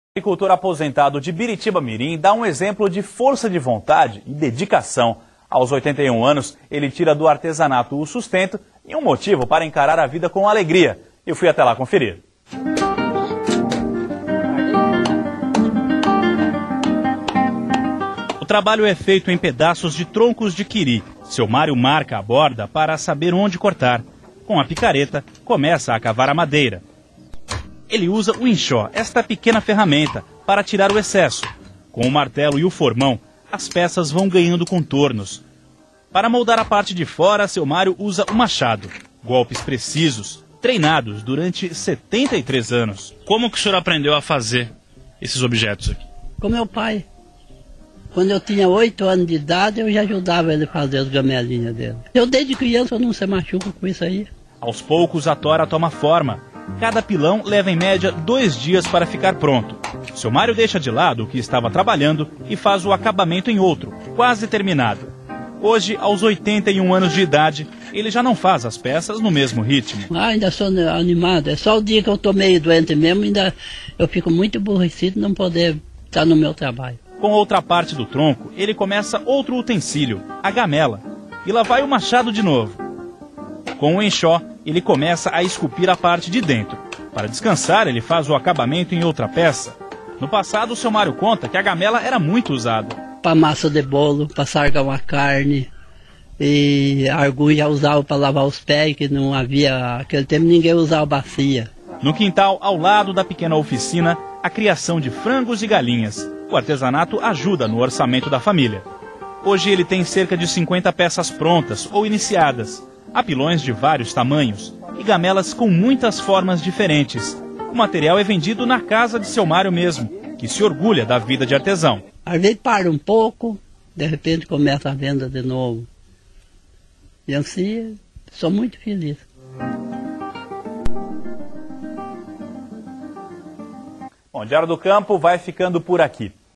Vídeo em homenagem ao Sr. Mario Pedro de Faria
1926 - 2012
Sr. Mário Pedro de Faria, este simpático artesão faleceu no dia 08 de março de 2012 aos 86 anos de idade vítima de câncer.
"Sr. Mario tinha 81 anos quando foi gravada esta reportagem"